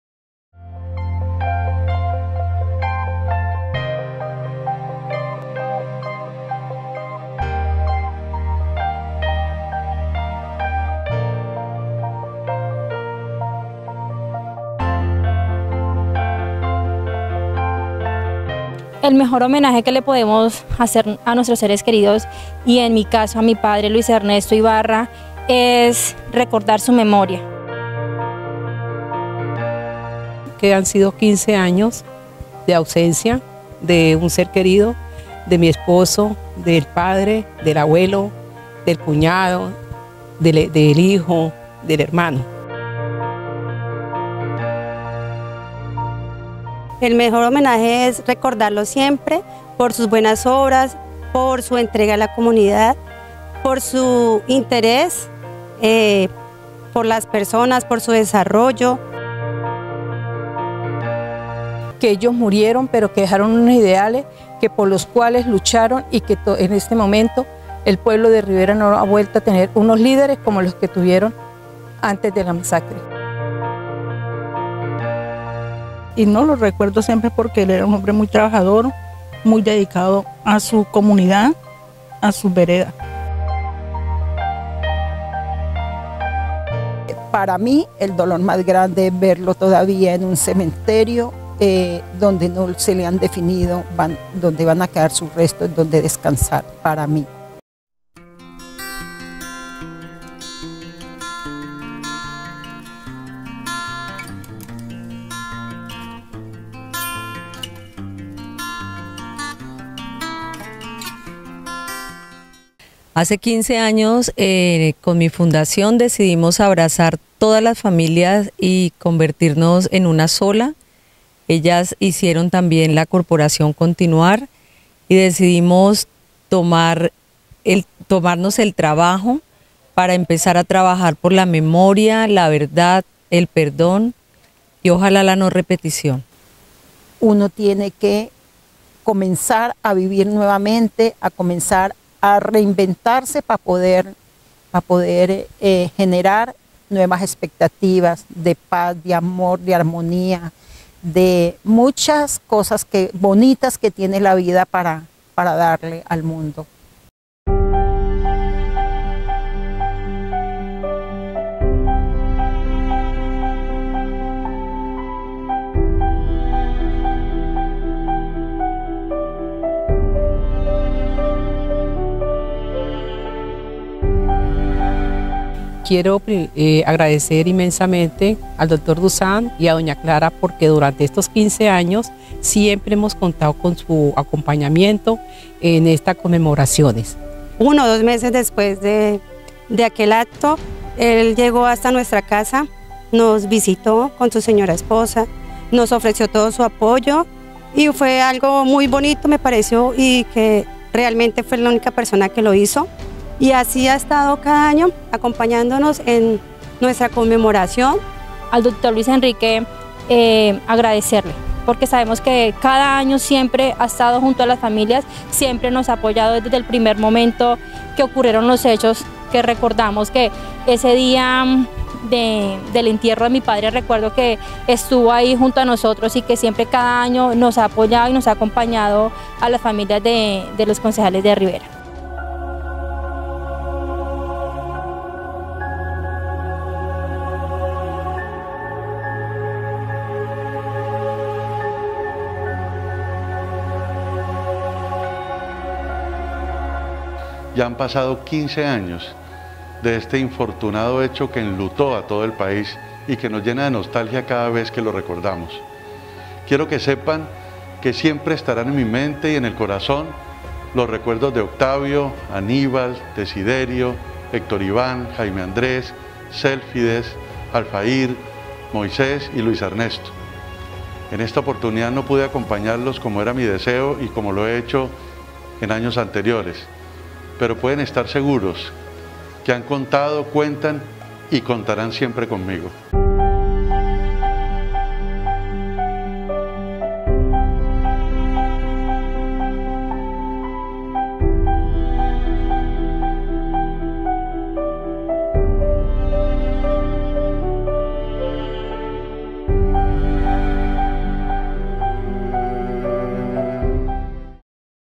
Homenaje a las familias de los concejales asesinados en Rivera
El mandatario de los huilenses Luis Enrique Dussán López, le envío un emotivo mensaje a las familias de quienes perdieron a un ser querido en la masacre perpetrada por las Farc contra los concejales del municipio de Rivera hace 15 años.

El gobernador Luis Enrique Dussán López le expresó a las familias de los concejales de Rivera que en su mente y corazón conserva los recuerdos de cada uno de estos líderes que pese a las amenazas siguieron ejerciendo su labor en favor de la comunidad y en un acto deplorable fueron asesinados hace 15 años por la desaparecida guerrilla de las Farc.